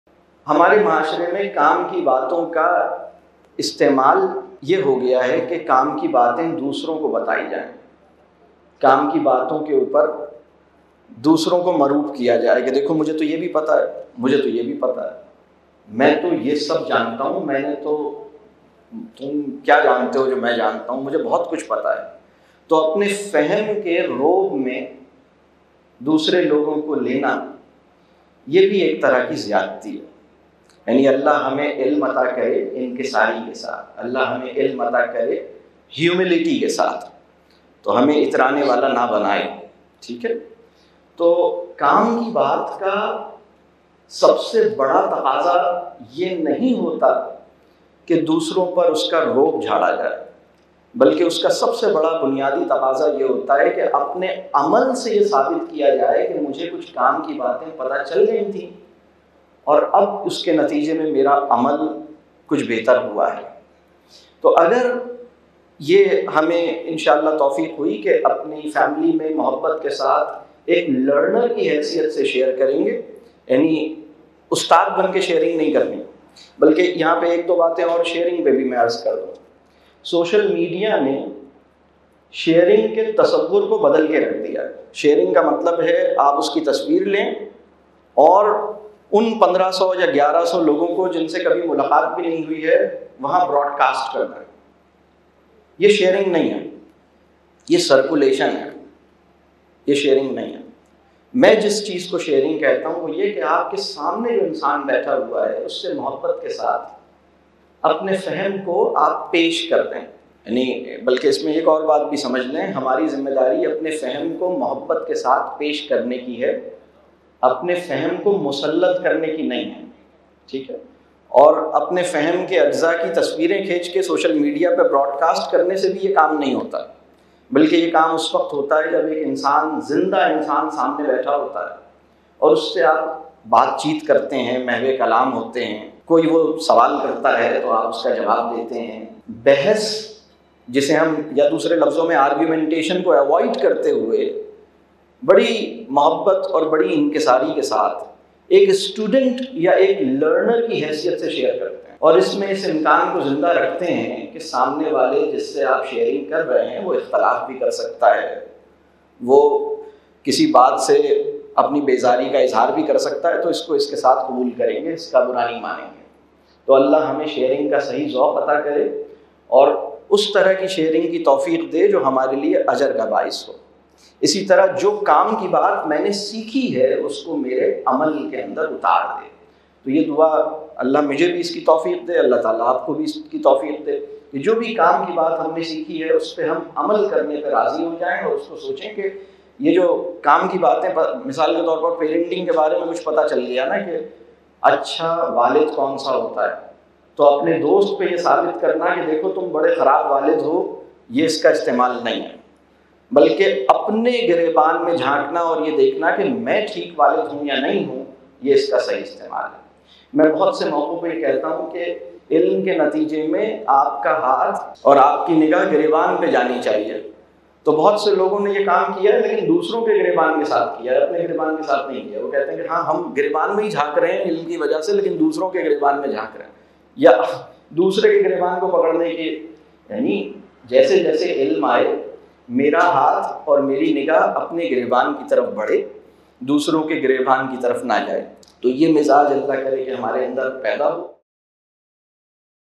How to Share Knowledge | Salman Asif Siddiqui
In this video, Salman Asif Siddiqui talks about how knowledge is meant to be shared.
This video is an excerpt from a longer program called 'Parenting & the Meaning of Life".

ERDC envisages social excellence through making teachers, learners, parents and schools realize their meaning and achieve their full potential.